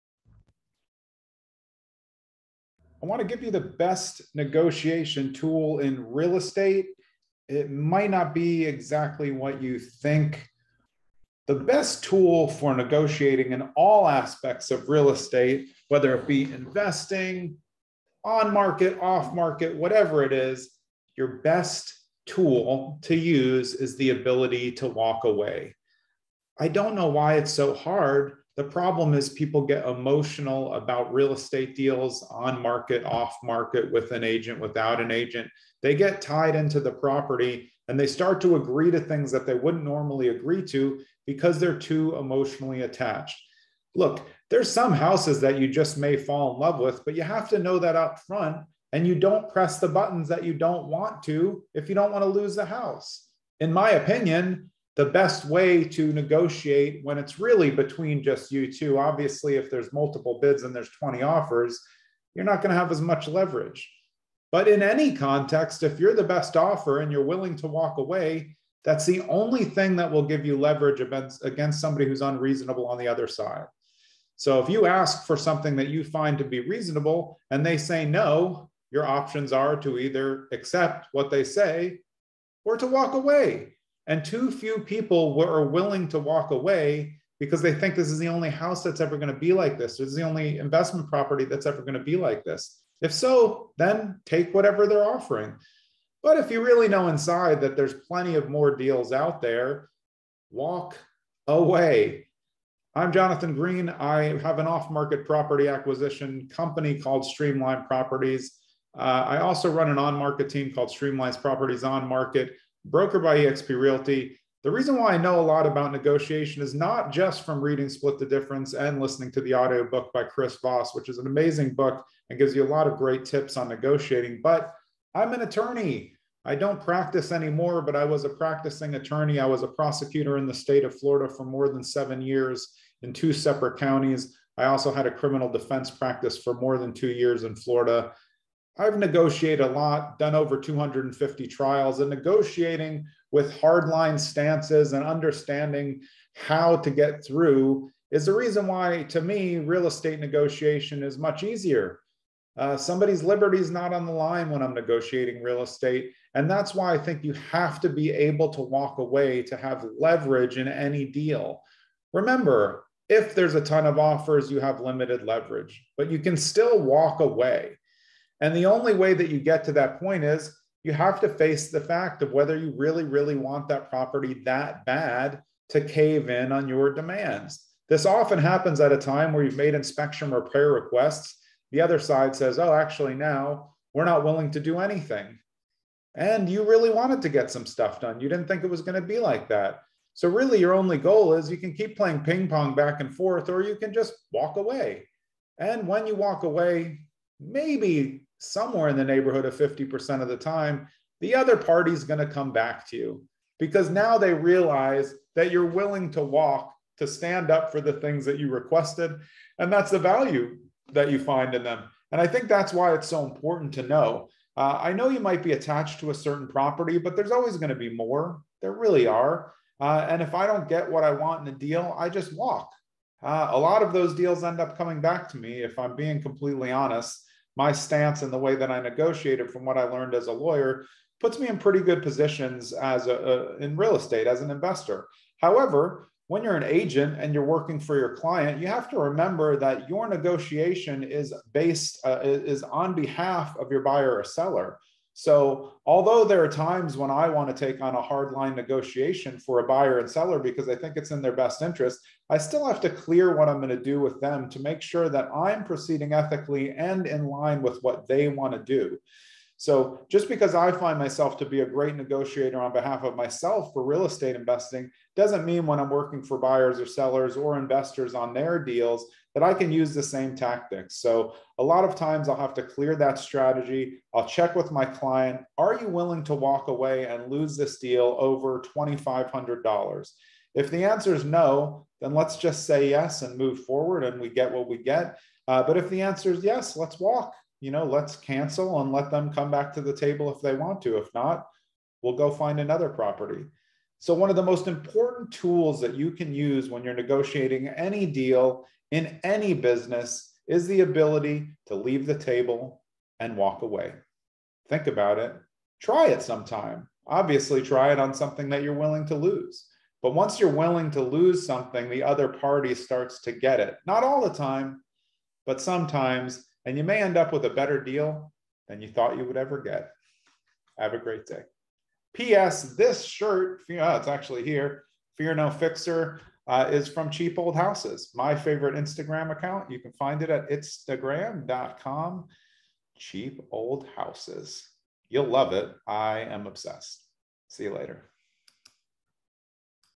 Why Walking Away is the Best Negotiation Tool in Real Estate
Negotiation in real estate gets corrupted by our emotional attachment to a certain property whether we are a buyer, seller, or investor. The best negotiation tool that I use in real estate is the ability to walk away. The other side has to know you are willing to walk away sometimes to understand that you were (and maybe still are) their best option.

I honed my negotiation skills as a prosecutor and criminal defense attorney in Florida for almost 10 years, which is why real estate negotiation seems easier to me. The stakes are high but much lower than liberty.